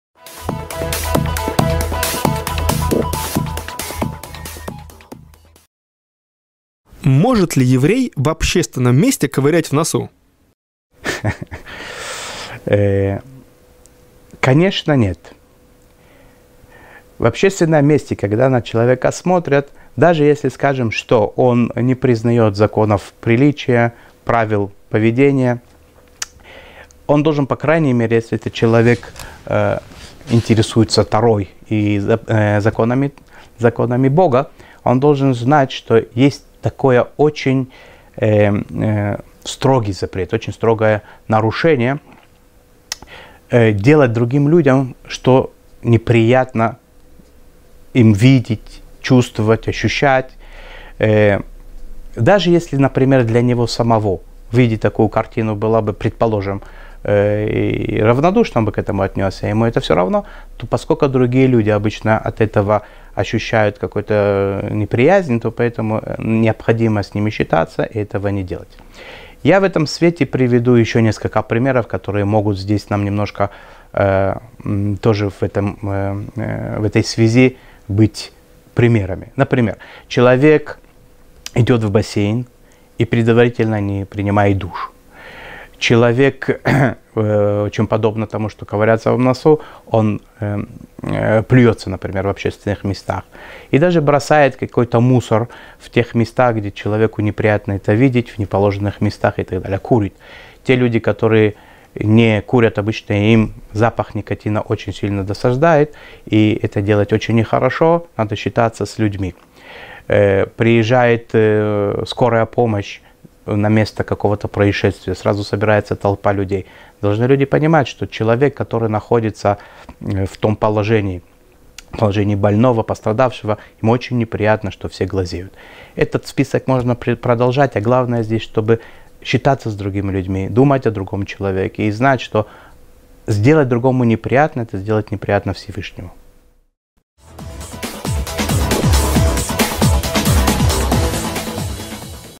Может ли еврей ковырять в носу? Ответ иудаизма за 3 минуты. Рав Лейб Нахман Злотник
🔹Может ли еврей в общественном месте ковырять в носу?
🔹Вроде бы ничего запрещенного тут нет, но как-то неприятно со стороны…